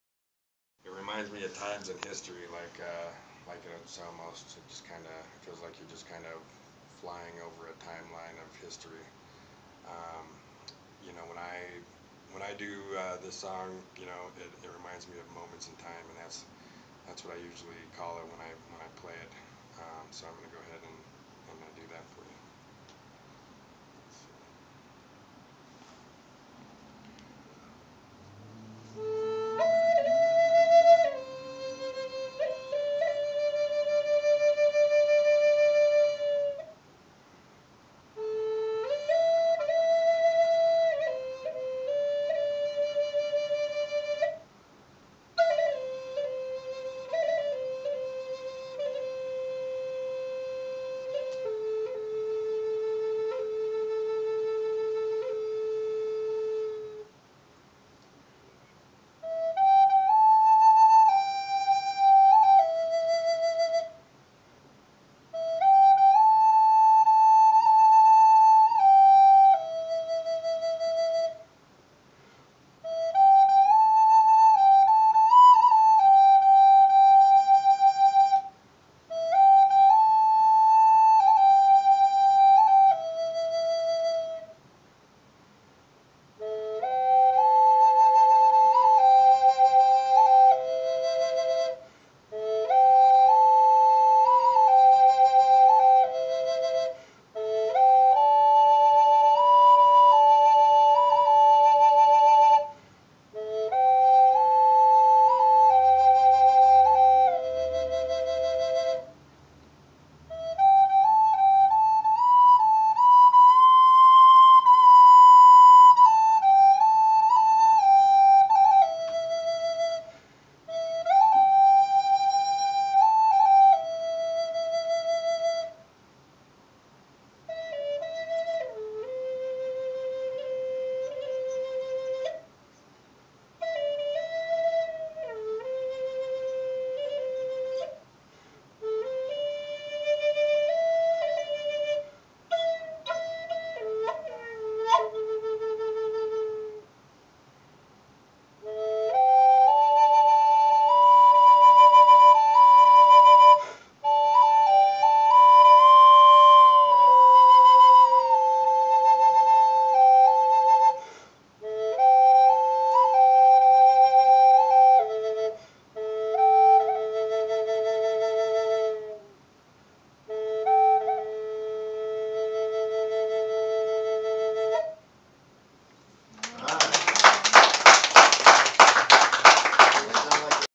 Moments in time
Dallas Brown live at the rescue haven for Utah women.